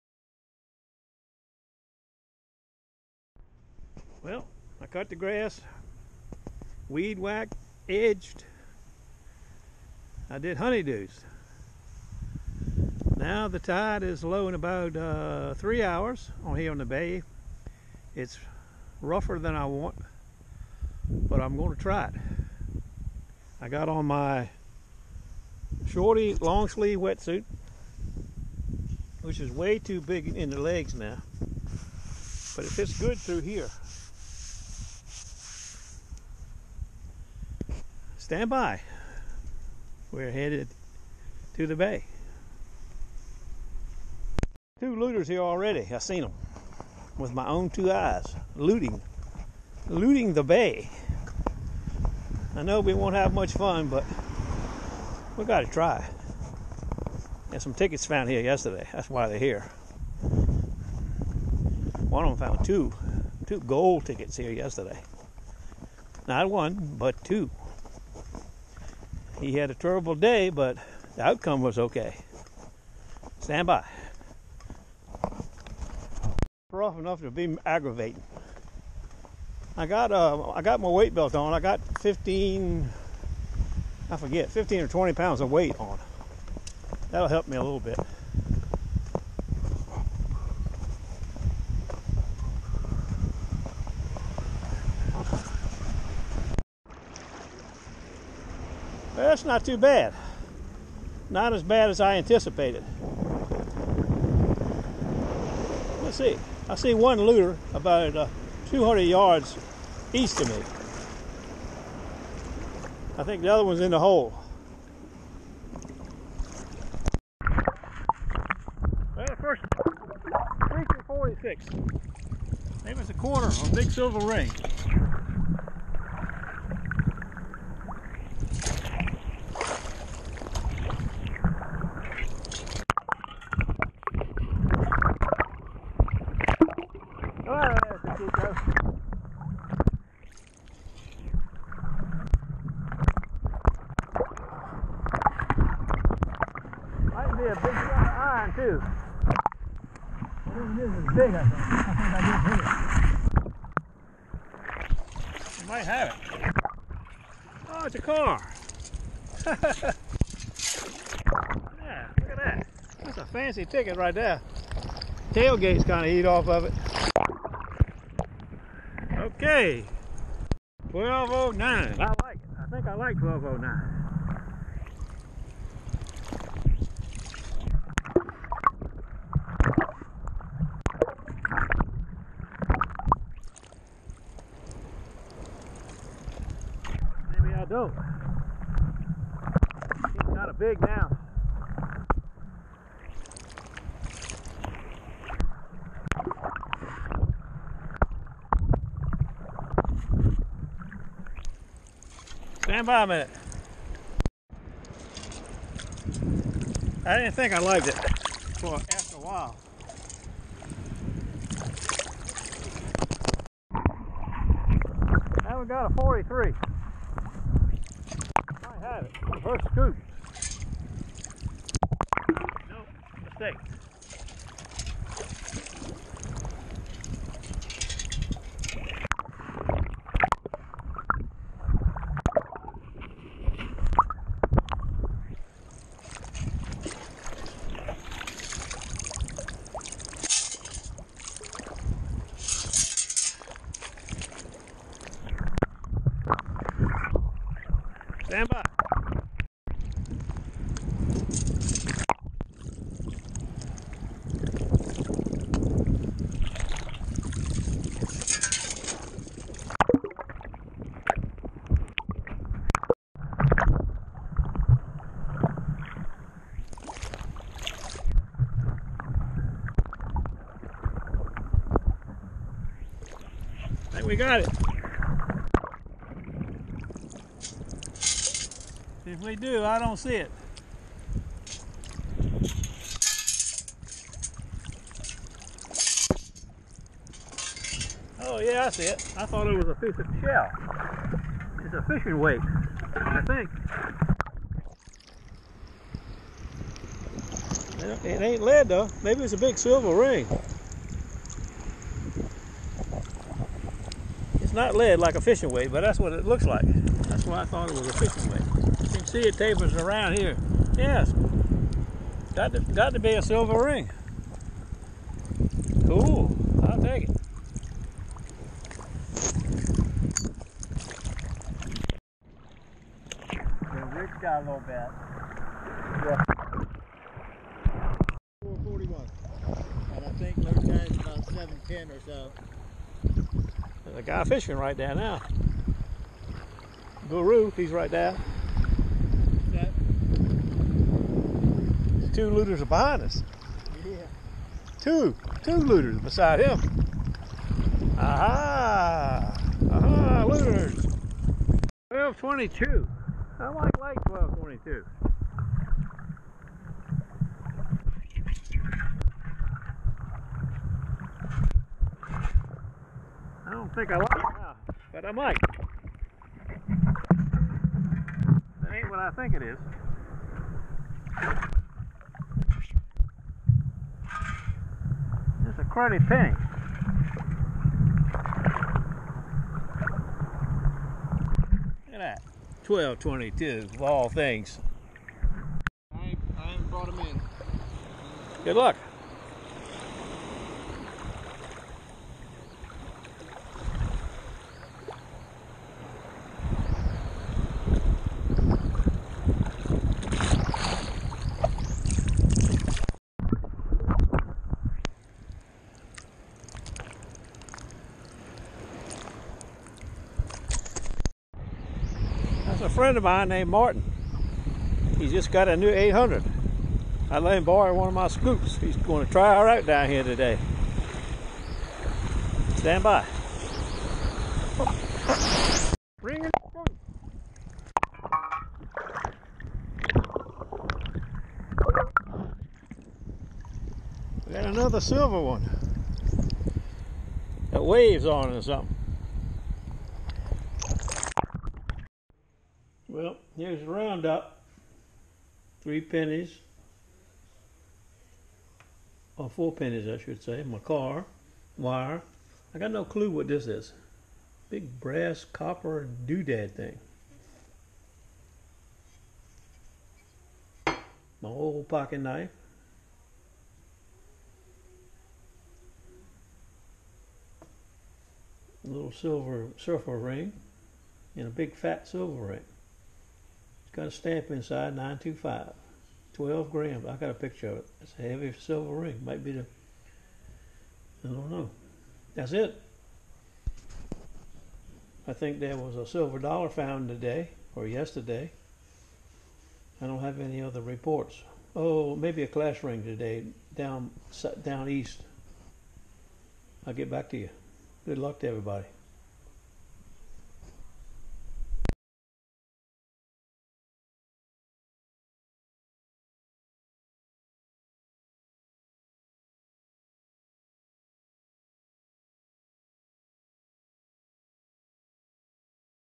surfchurch yields 12.2 + grams of silver today - Bay
conditions were not great, but had to try.

surfchurch spin was better than expected - see you monday.

Stavrscoop
go pro hero session camera